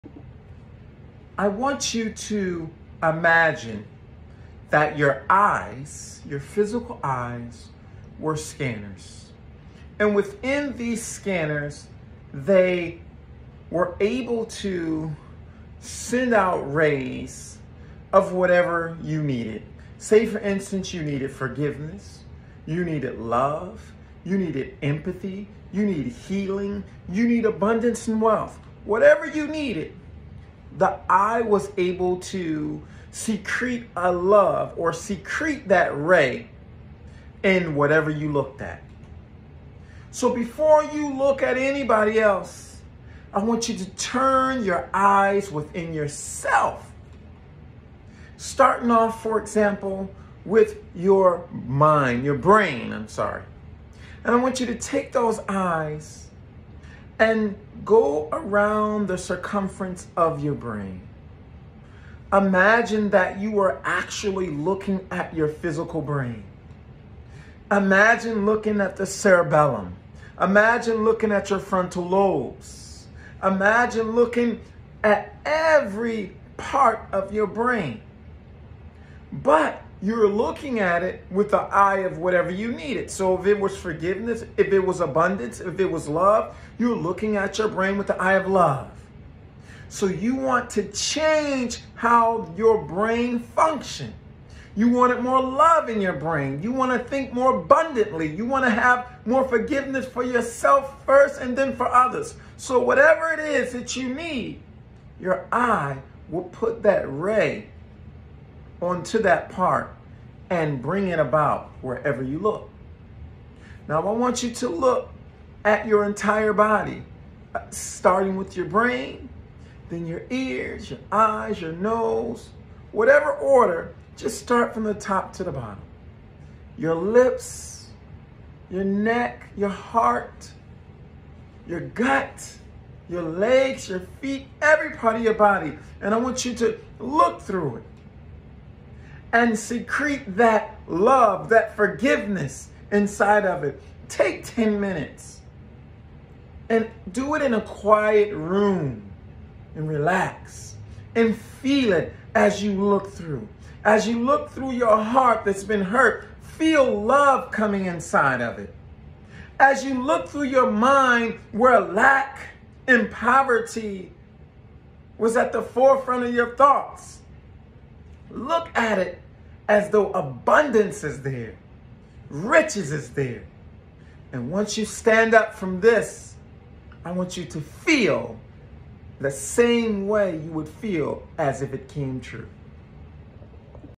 Imagination makes reality
Mind power, visualization, abundance thinking techniques that will help you rewire, and become the greater you.